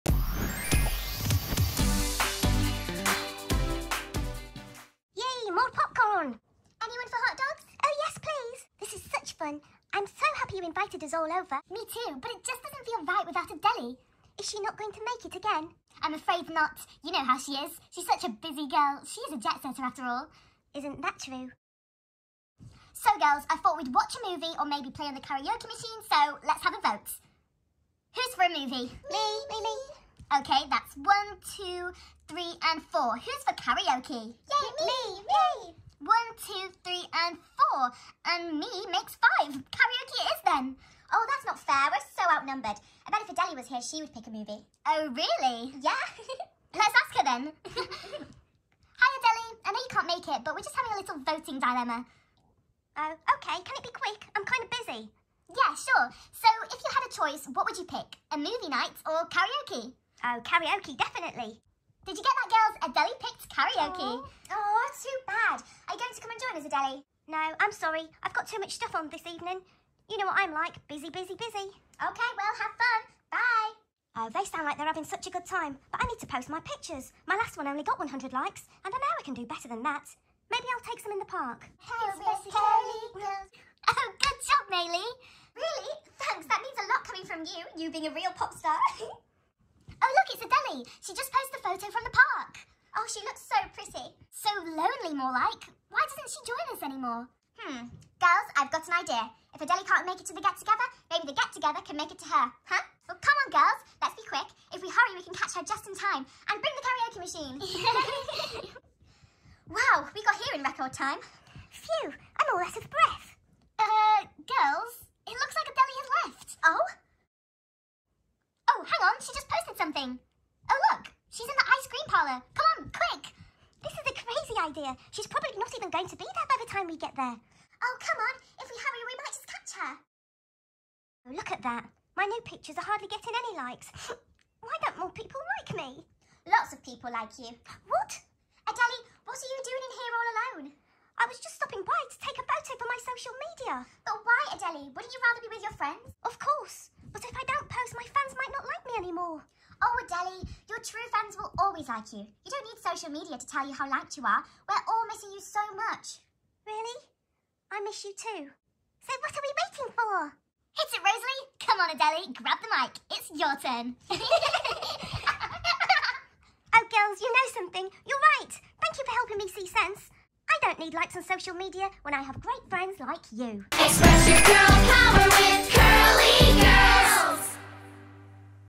Jet Setter's Best Curl Friends! A CurliGirls Story
Today we have another CurliGirls story! This time we are with Adeli the Jet Setter!

UK

US

Amazon